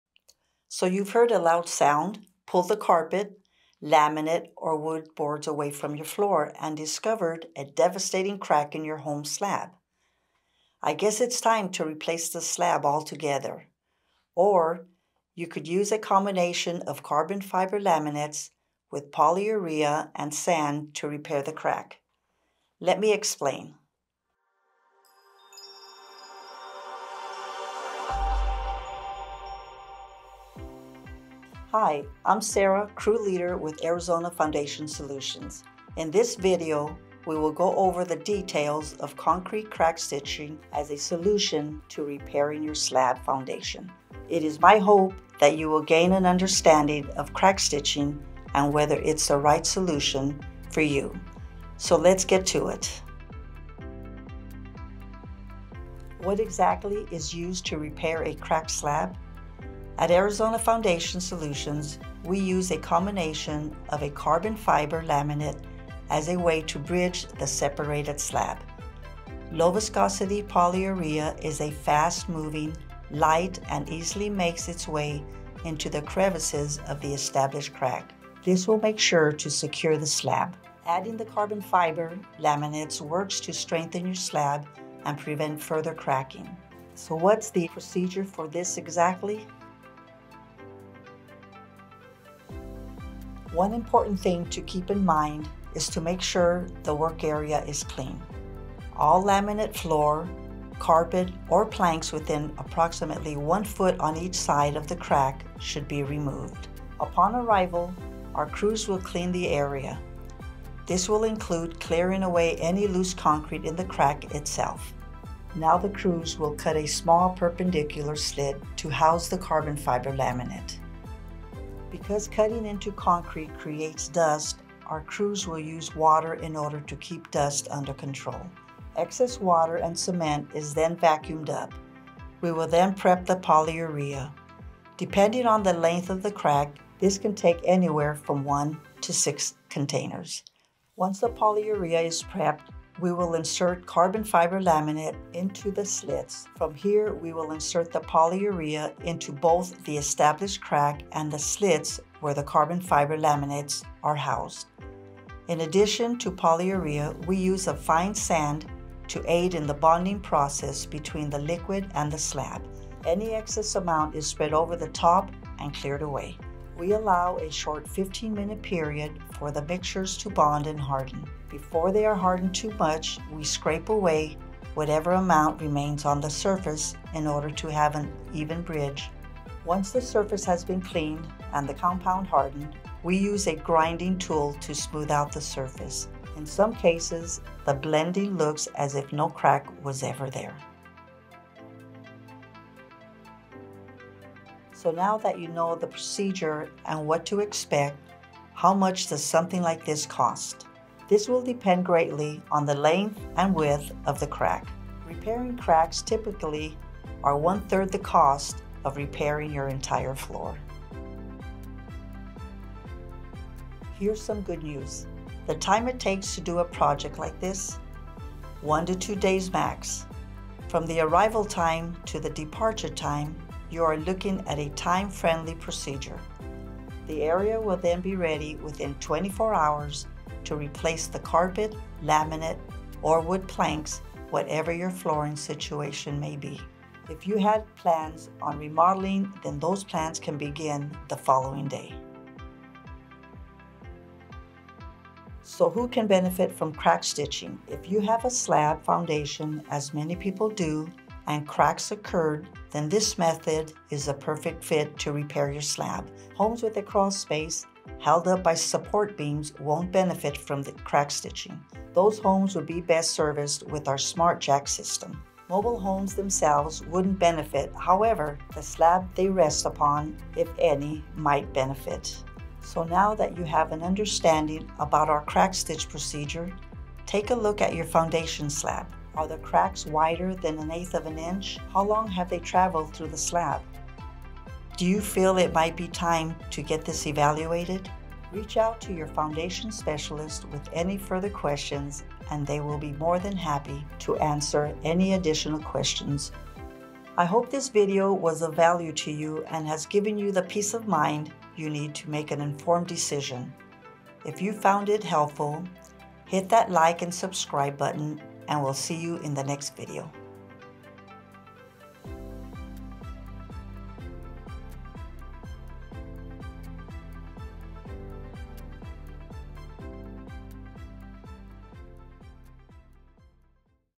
Why crack stitch concrete repair might be the right solution for your damaged slab.
Are foundation problems causing cracks in your floor? Need a concrete crack filler? Tune into this video and find out why crack stitch concrete repair might be the right solution for your damaged slab. Whether they are caused by foundation heave or settlement, by utilizing the incredible strengthening properties of Glass, Kevlar, and Carbon Composites, we can provide a cost-effective concrete repair solution for your concrete cracks.

Carbon fiber has been used in everything from the Space Shuttle to the Boeing 777 Dreamliners. These materials have made their way into mainstream construction and are creating numerous benefits including foundation crack repair and much more!

00:00-00:53 Intro
00:53-01:29 Features
01:29-03:10 Installation
03:10-03:32 Cost
03:32-04:02 Timeline
04:02-04:32 Who it's for & not for
04:32-05:10 Conclusion